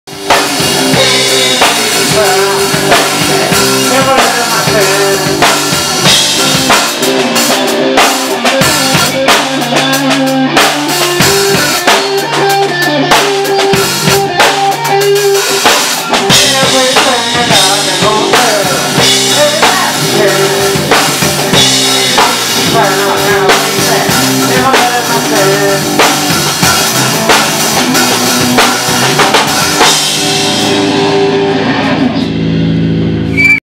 Killing Sunday
Killing Sunday live at The Rebel Lounge in Phoenix, AZ on 6/27/19